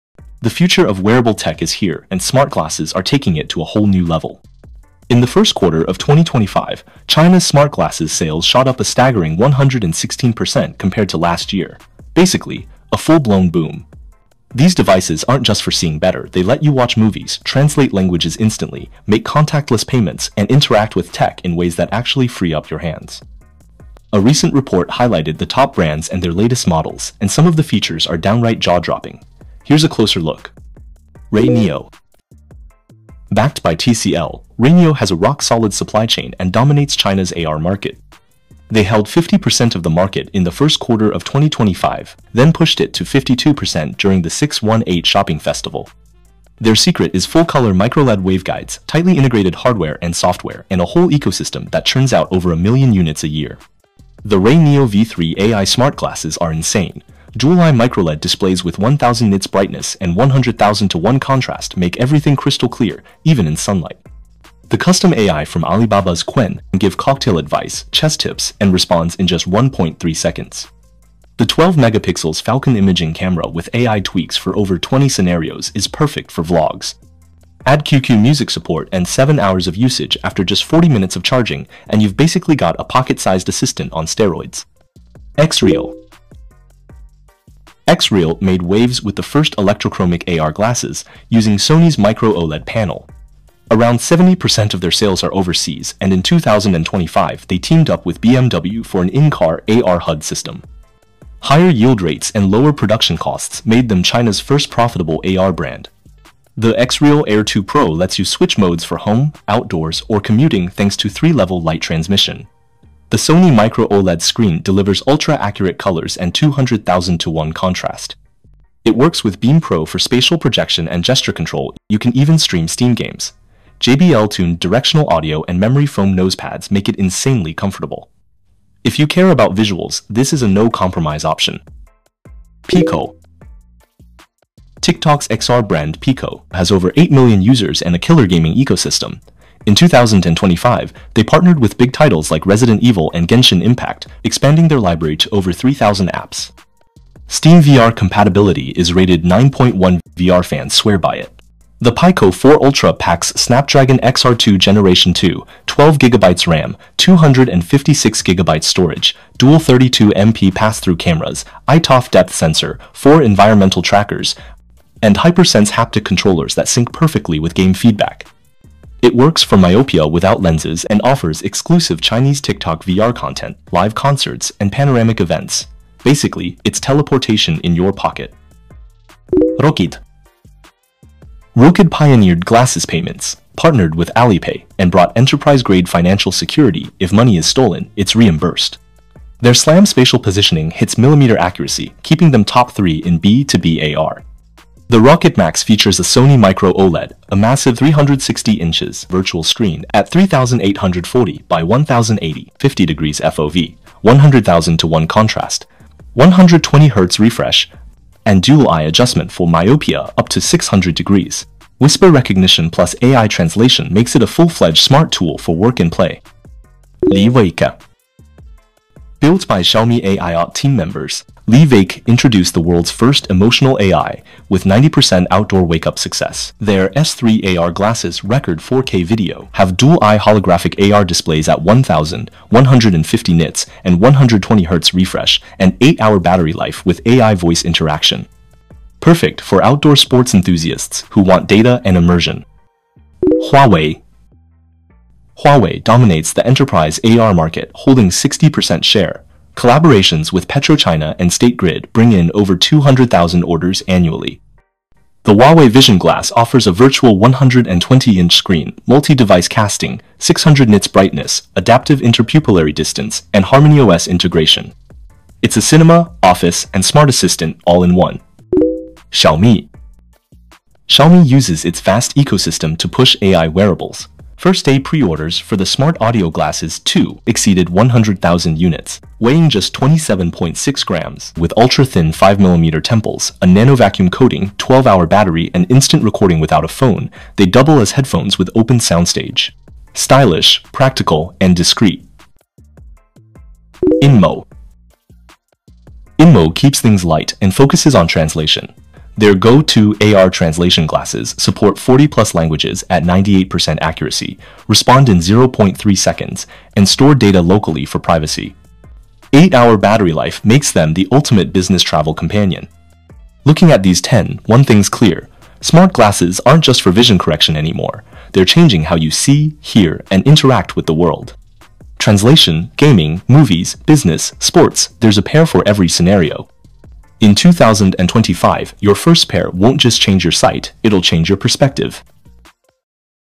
INSANE China Smart Glasses You Must See in 2025
Smartphones are old news—2025 is all about smart glasses. 👓🔥
China’s wearable tech scene is exploding, with sales up more than 116% this year. From AR gaming that feels like teleportation to glasses that translate 40+ languages in real time, the future is literally right in front of your eyes.

In this video, we’re diving into the most insane Chinese smart glasses of 2025—RayNeo, XREAL, PICO, Rokid, Huawei, Xiaomi, and more. Some of these look straight out of a sci-fi movie, and others might just replace your phone.